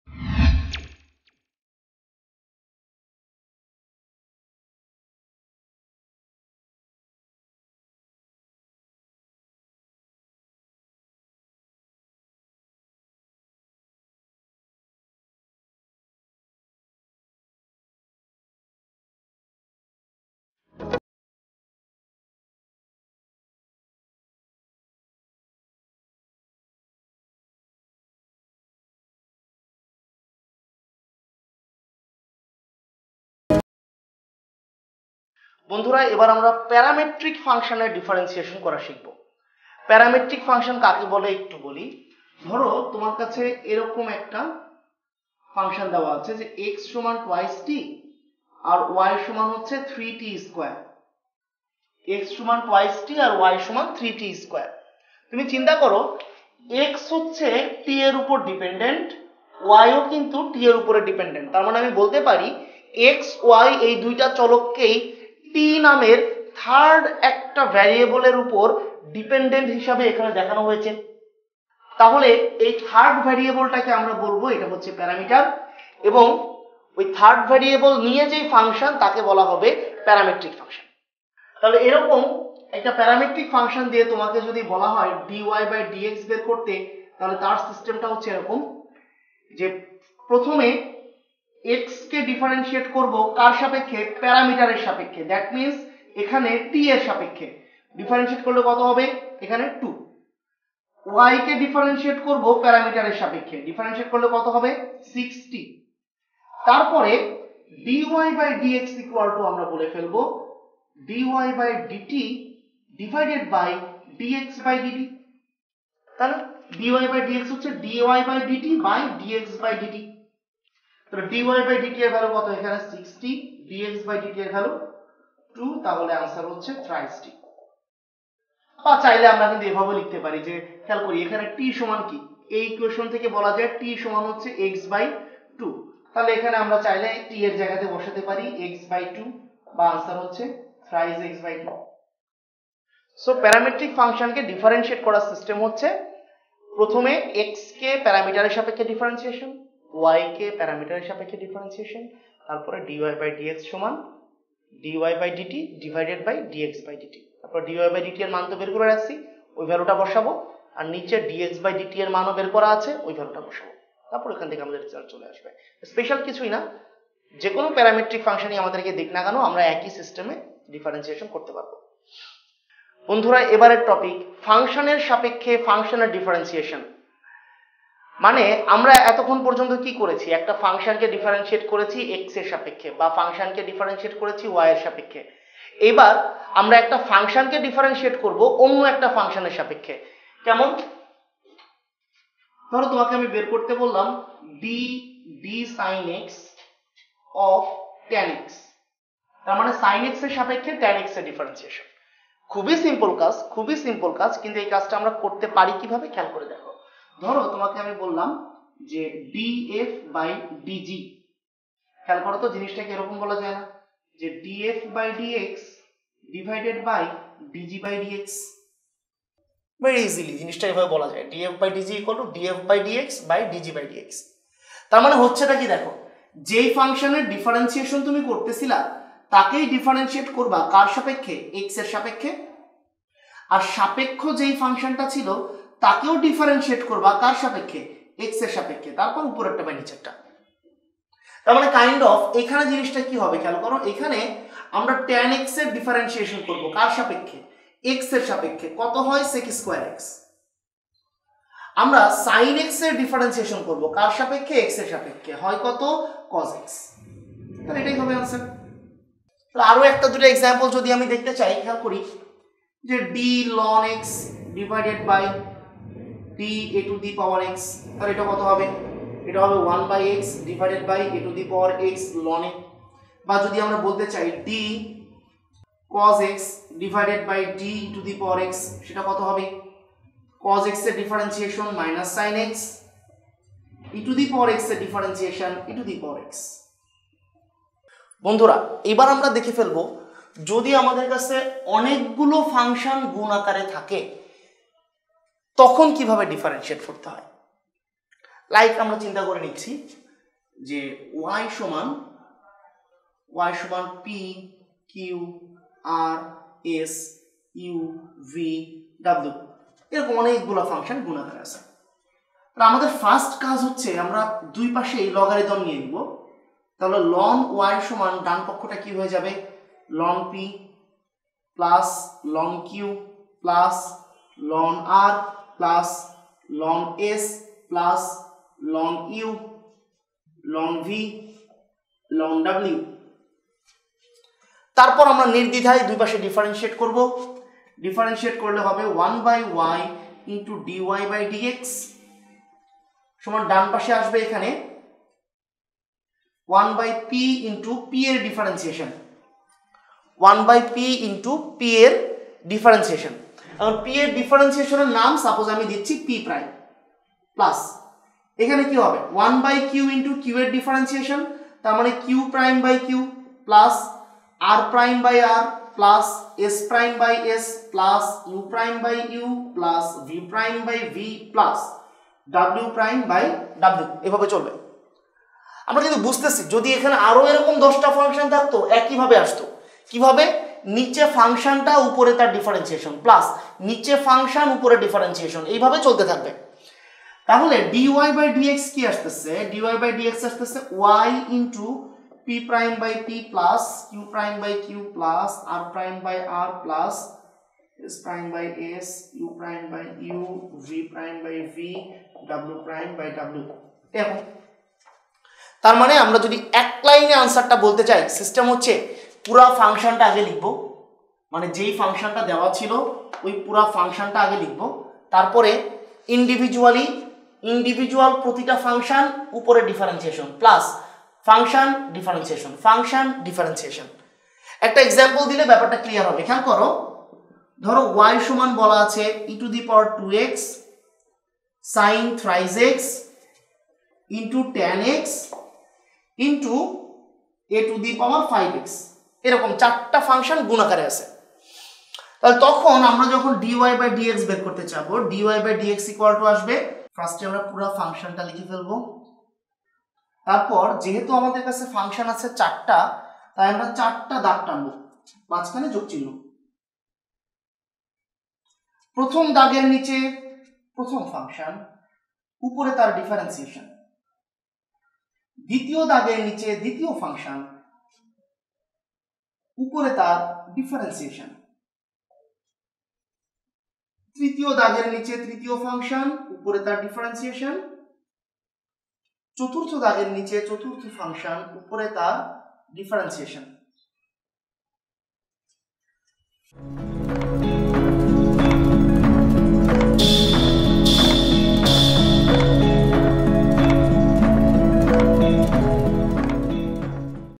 10. প্যারামেত্রিক ফাংশন এবং ফাংশনের সাপেক্ষে ফাংশনের অন্তরীকরণ | OnnoRokom Pathshala
Video Title: OnnoRokom Pathshala_Math
Lecture: Differentiation Lecture-01
Subject: Math
Topic: Parametric Function & Differentiation of this Function with Respect to Function
Class: HSC 1st Year
Lectured by: Ashikuzzaman Rasel